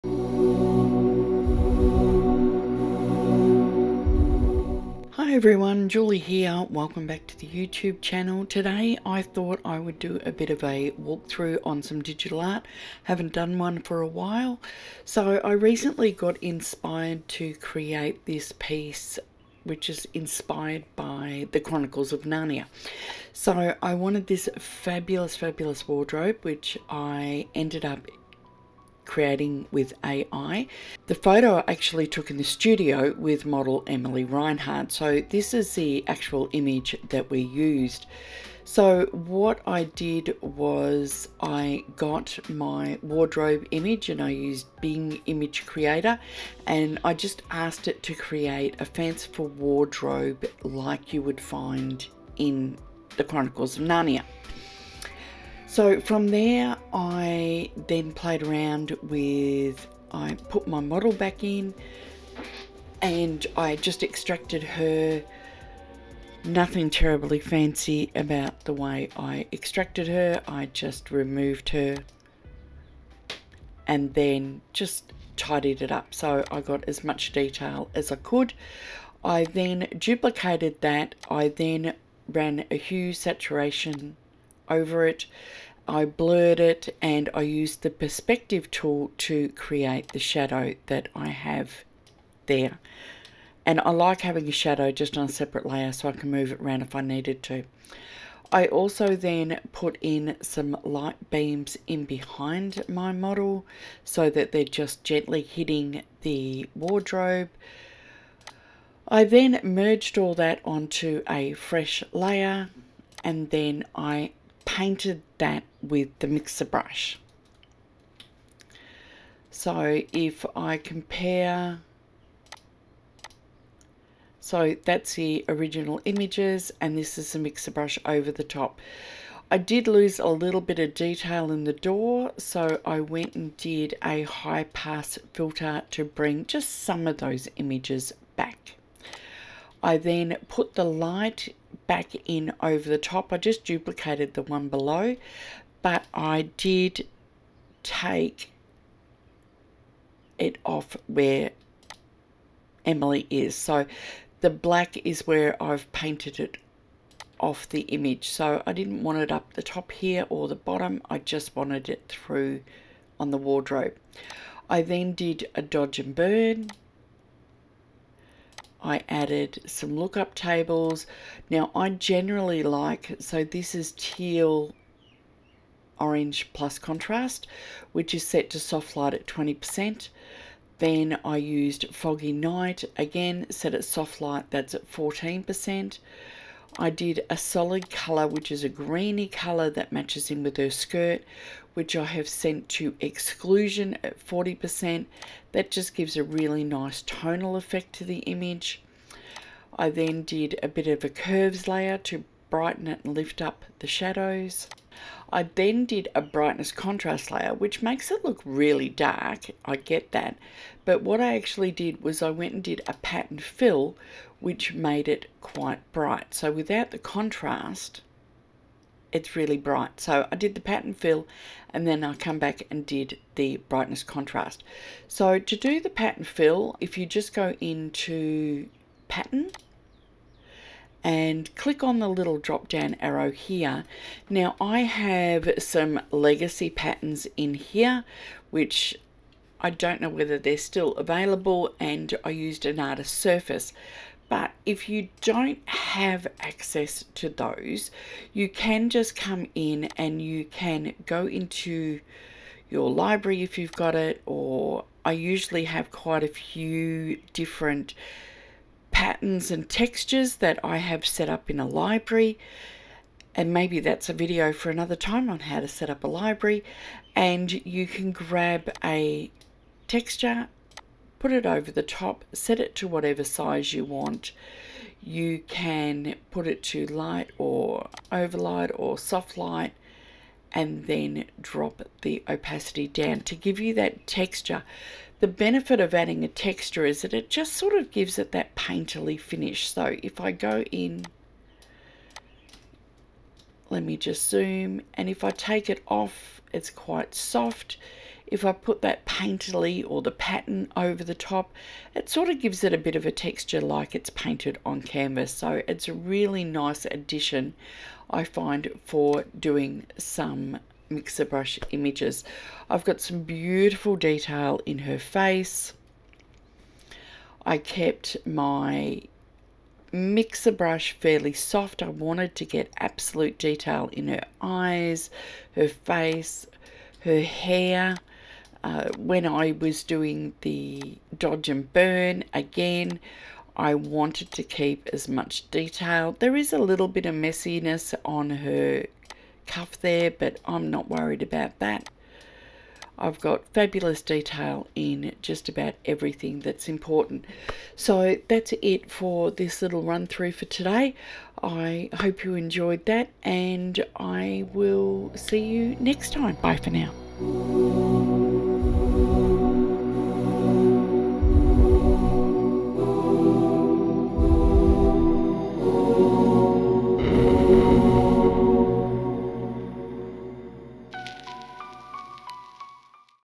Narnia Walkthrough
I created this image - Magical Doorway to Narnia from a composite part AI wardrobe image (I don't have any fanciful wardrobe images in my collection yet! And a studio portrait of model Emily Reinhard of a Narnia-inspired series inspired by Susan Pevensie's character from The Chronicles of Narnia. After I created the composite I then painted it with the mixer brush (about 5 hours to paint), added a texture and some colour toning and voila! Finished piece.

If you would love to learn how to paint with the mixer brush, why not check out my Master the Mixer Brush self-paced online class

To create my digital art I used the following:
Huion Kamvas Pro 16 Pen Display